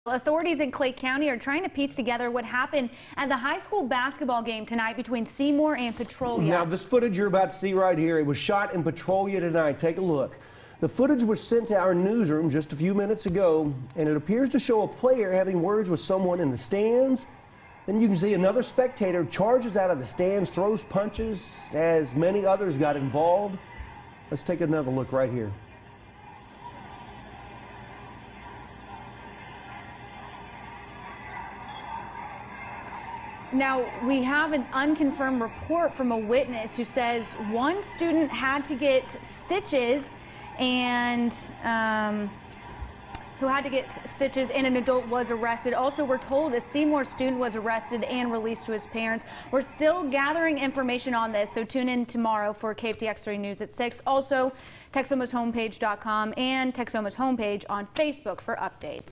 Fight breaks out at Petrolia basketball game
Authorities in Clay County are trying to piece together what happened at the high school basketball game tonight between Seymour and Petrolia.